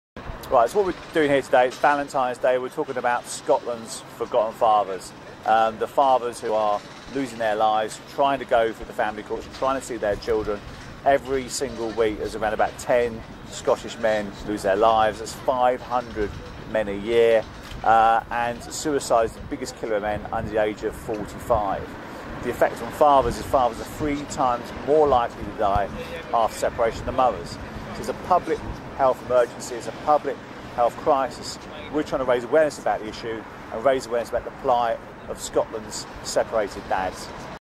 Fathers4Justice launch the "Forgotten Fathers" campaign outside the Scottish Parliament
Matt O'Connor and Fathers4Justice launch the "Forgotten Fathers" campaign outside the Scottish Parliament with MSPs, Bill Kidd (Scottish Government Chief Whip) and Fin Carson.

The event attracted widespread media coverage on Scottish television, radio and in the press including The Scottish Sun, The Scotsman and The Sunday Post.